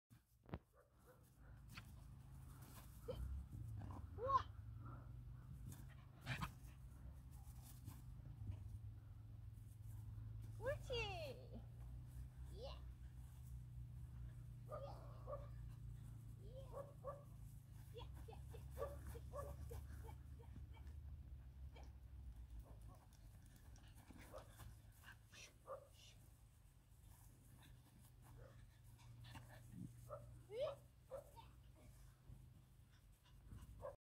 장난감 공보다 터그놀이 즐기는 루이
장난감 공보다 터그놀이 더 좋아하는 루이
오늘은 8코기 보다가 루이영상 짤막하게~^
일요일은 루이랑 하트섬 산책하는 날~^
아무도 없이 둘이서 신나게~^^

터그놀이 좋은걸루 장만해야겠네요~^